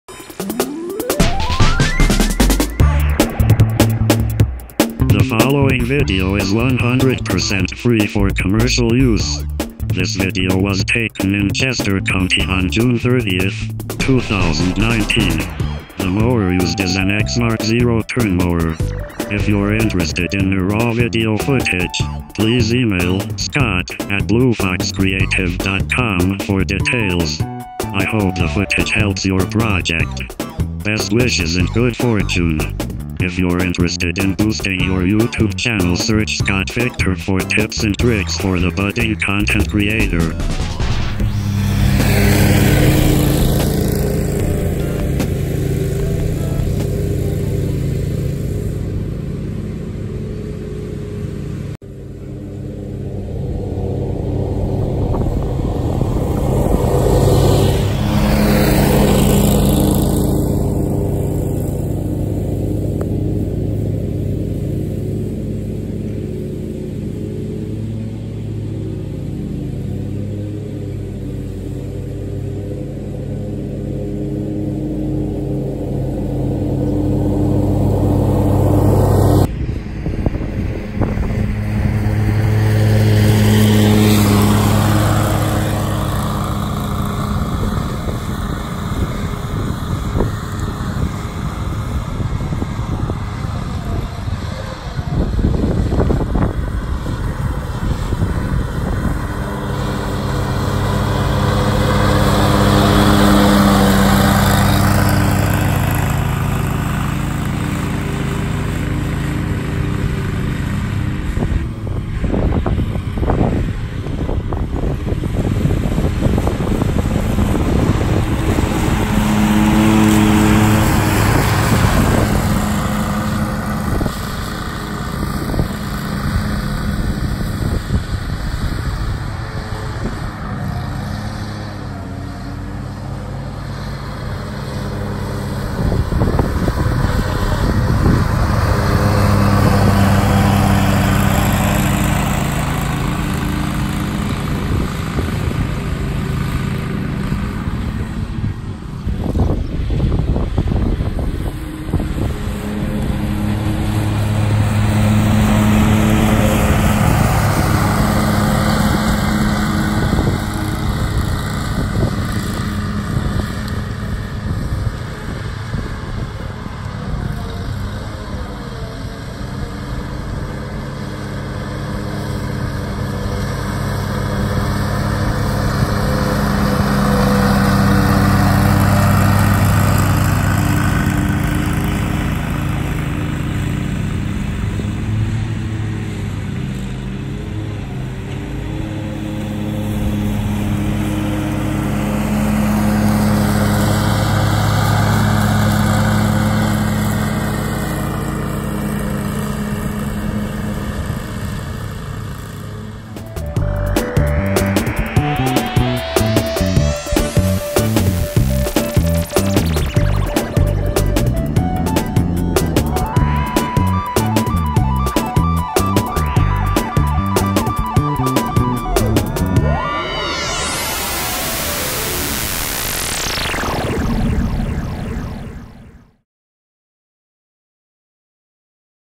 Free For Commercial use Exmark Zero Turn Lawn Mower Video
This is a 100% free for commercial use video showing a person mowing the lawn with a Exmark Zero Turn mower.